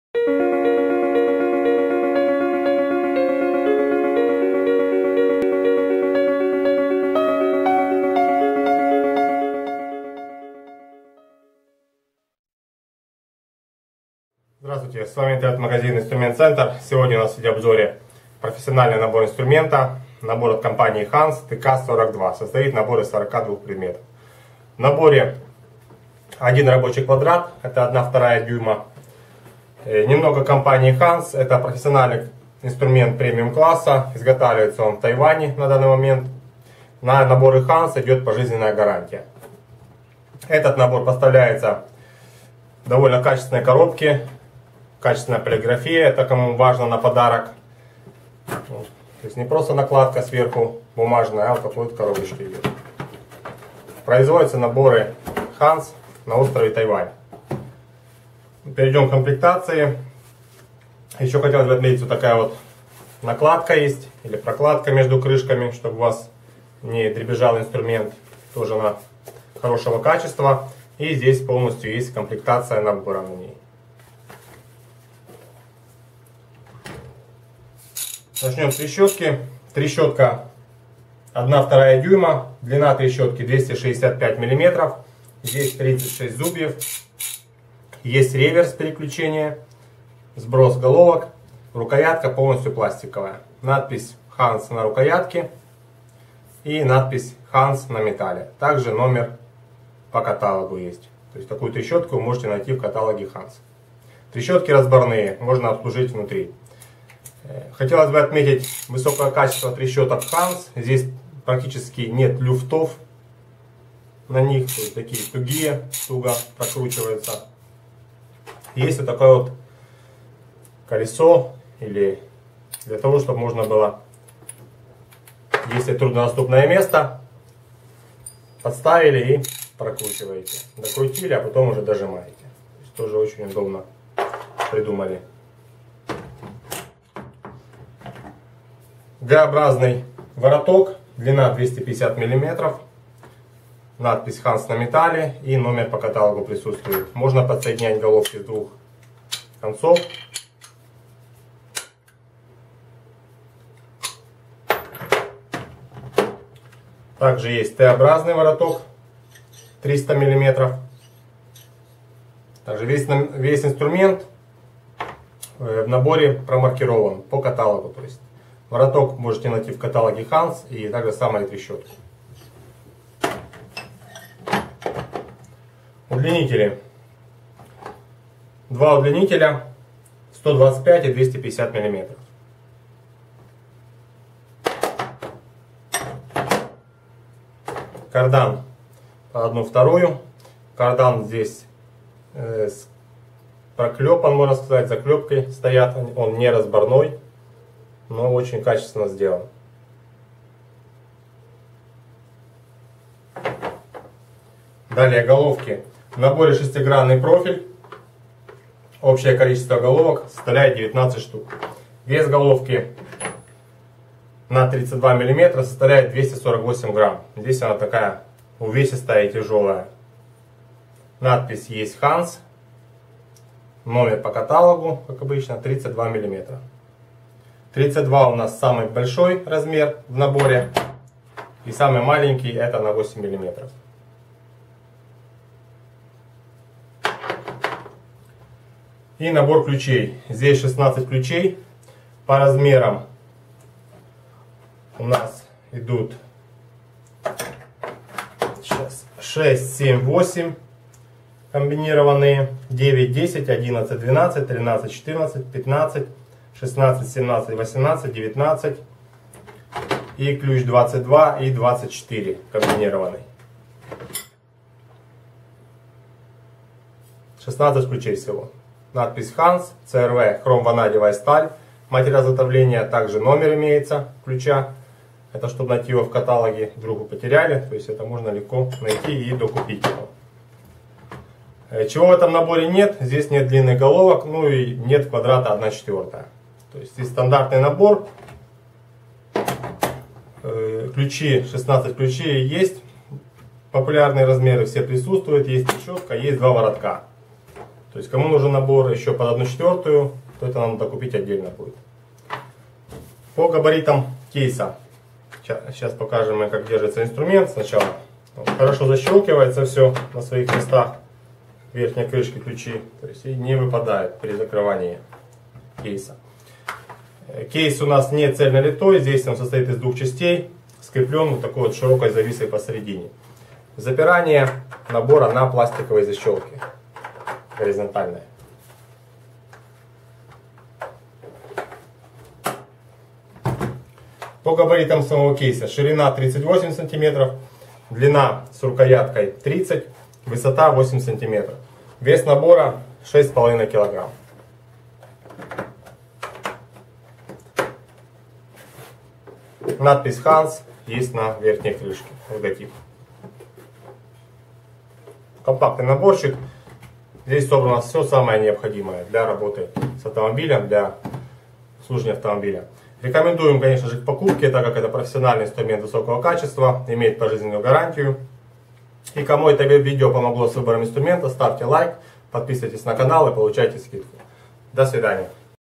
Набор инструментов HANS TK-42 (42 предмета)
Набор инструмента HANS TK-42 имеет хорошую комплектацию по головкам(до 32 мм) и ключам(до 24 мм). Набор очень компактный. Особенностью набора является высочайшее качество и прочность металла. Изготовлен из первоклассных сплавов хром ванадия и хром молибдена, которые делают набор очень качественным и крепким. Инструменты HANS работают с повышенными нагрузками, относится к классу индустриального инструмента.

Комплектация:

Головки торцевые 1/2: 10,12,13,14, 15,16,17, 18,19,20, 21, 23, 24, 27, 32,
Удлинители 1/2: 125 мм, 250 мм
Вороток 1/2”: 300 мм
Карданный шарнир 1/2”, 70 мм
Трещотка с быстрым сбросом 1/2”
Ключи комбинированные: 6,7,8,9,10,11,12,13,14,15,16,17,18, 19, 22, 24 мм
L-образный вороток 1/2”
Диск зубчатый 1/2”